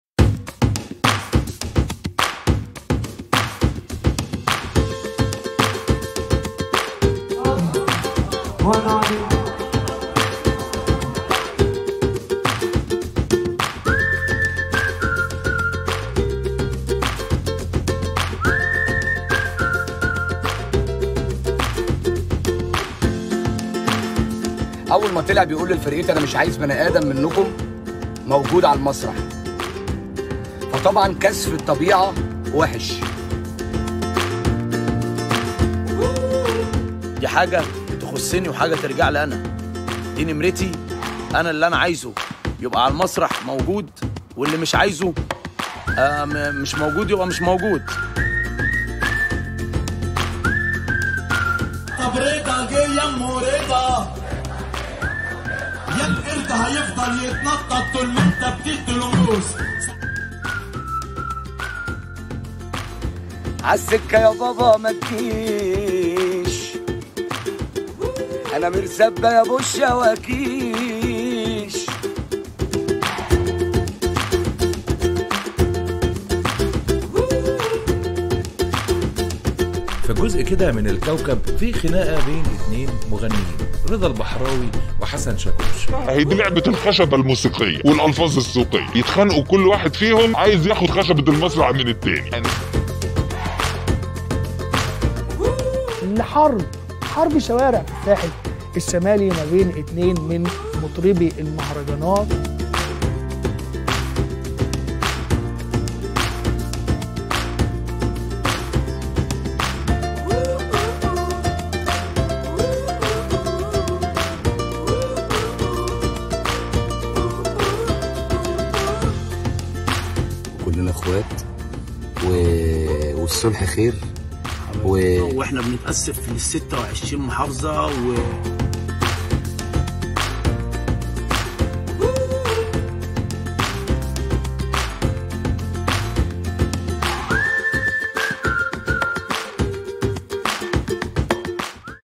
Deux chanteurs égyptiens se disputent sur scène 👀
Égypte : une dispute entre deux chanteurs de "Mahraganat" fait le buzz. Regardez jusqu'à la fin 👀